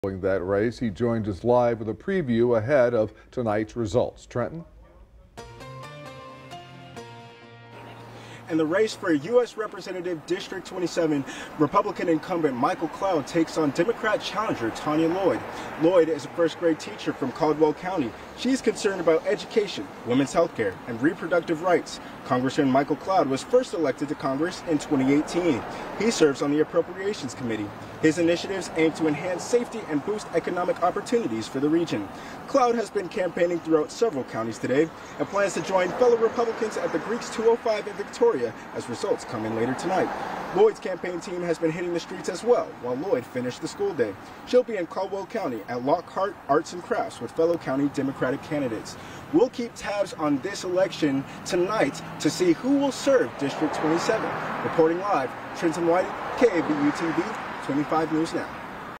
Cloud vs Llyod battle for District 27
Republican Michael Cloud and Democrat Tanya Lloyd battle for District 27